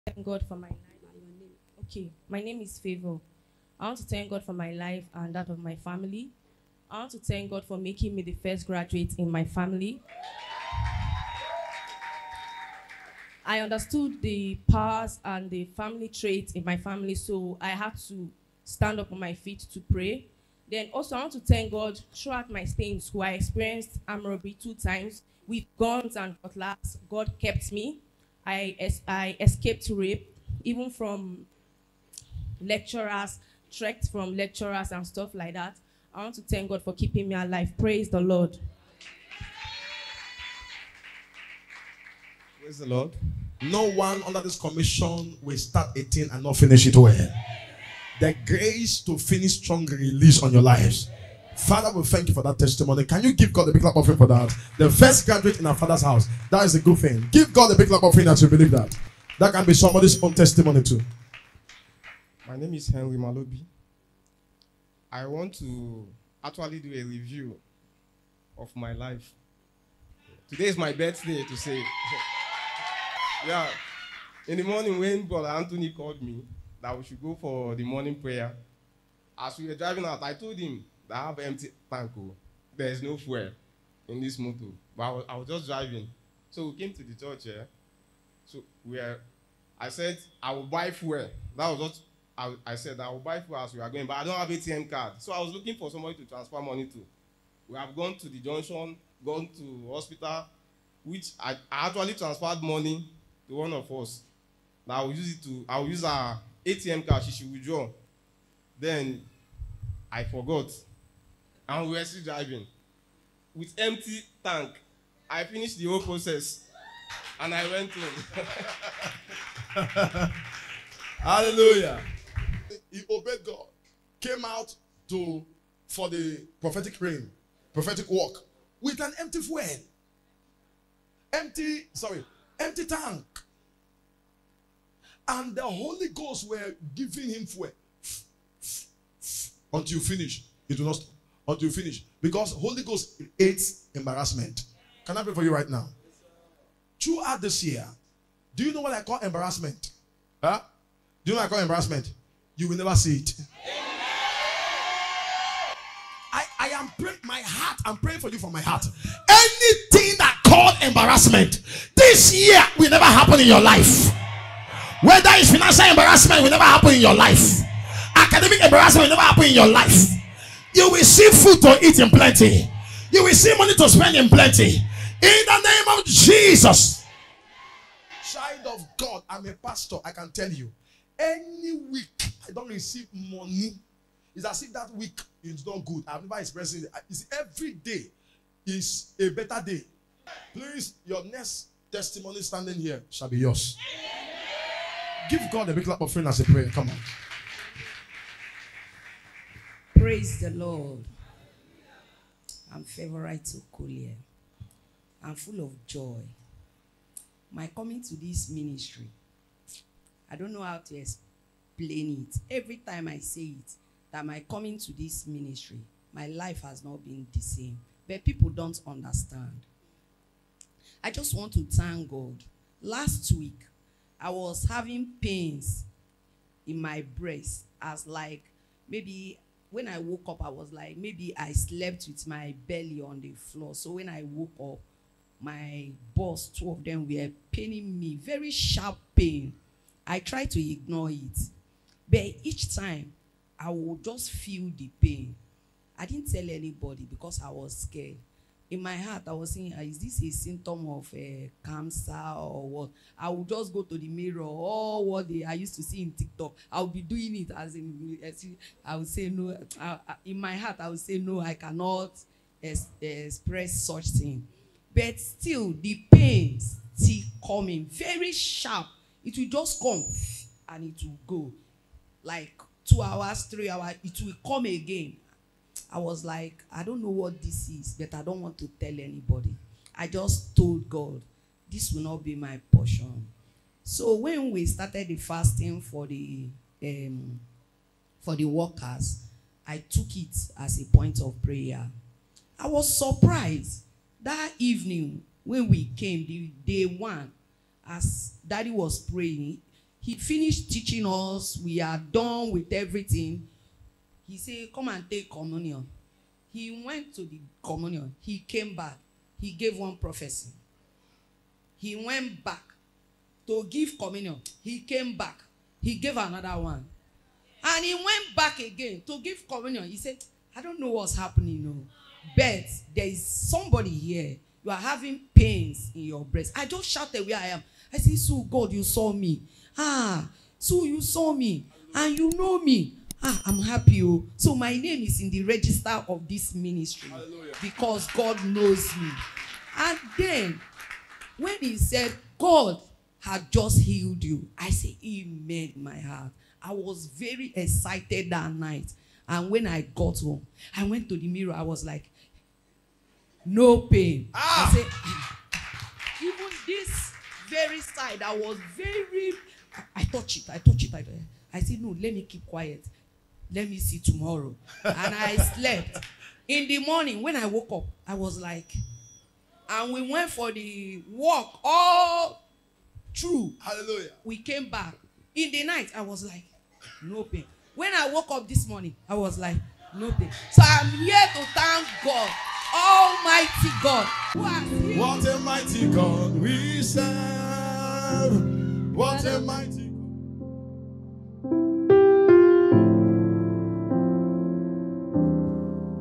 I DROVE MY CAR WITHOUT FUEL FOR MORE THAN 3 HOURS || FLOURISHING TESTIMONIES || JANUARY 14TH 2024
FLOURISHING TESTIMONIES || THE REFORMER'S HOUSE INT'L CHURCH || JANUARY 14TH 2024
Pain in the breast disappeared by the powerful hand of God.